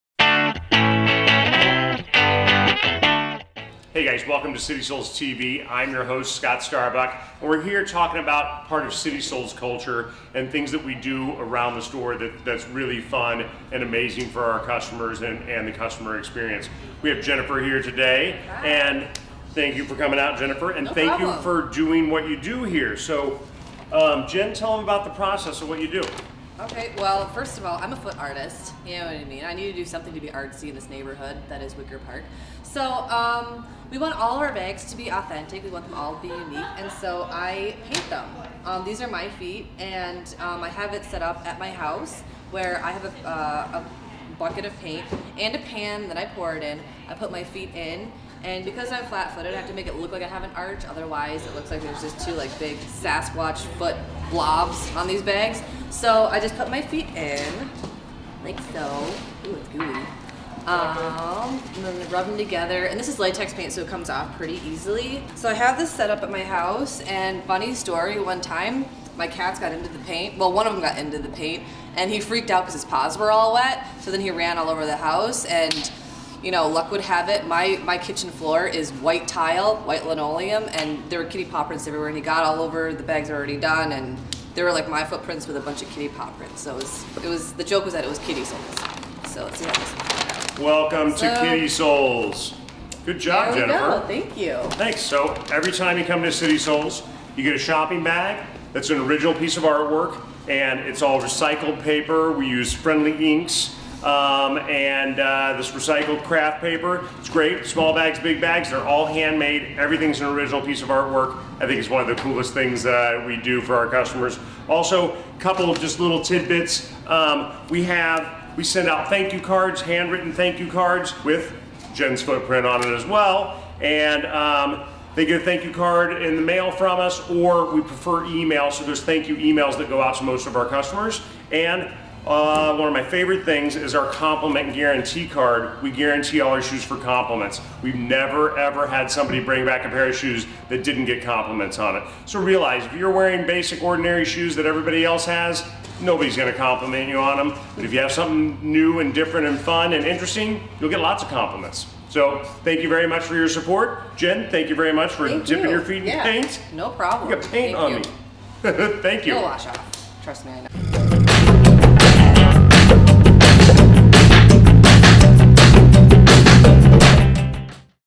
CS Shopping Bags How-To with Jennifer | CitySolesTV - Episode #104
Hey guys welcome to City Soles TV. I'm your host Scott Starbuck and we're here talking about part of City Soles culture, including fun things we do around the store for our customers!